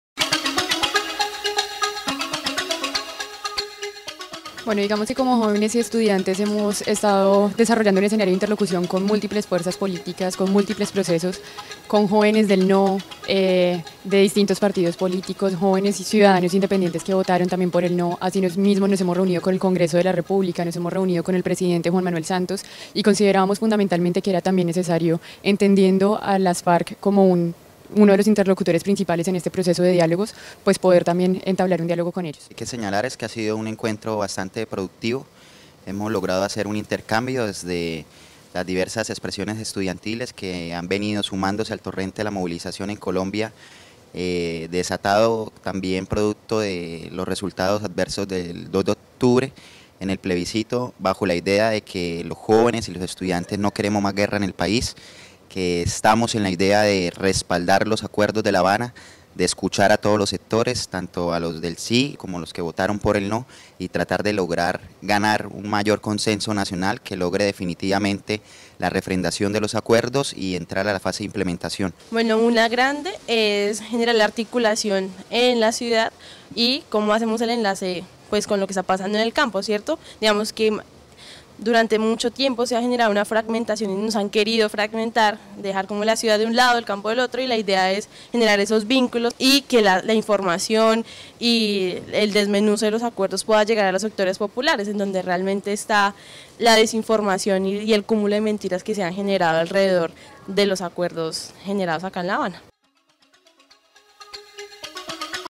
Líderes y lideresas estudiantiles intercambian ideas con delegados de Paz de FARC-EP
Líderes y lideresas estudiantiles intercambian ideas con delegados de Paz de FARC-EP en La Habana, Cuba. Entre las propuestas a destacar fue el "Manifiesto de Acuerdo YA" carta de los estudiantes a la paz que salió como conclusión del "Encuentro Nacional de Universidades por la Paz"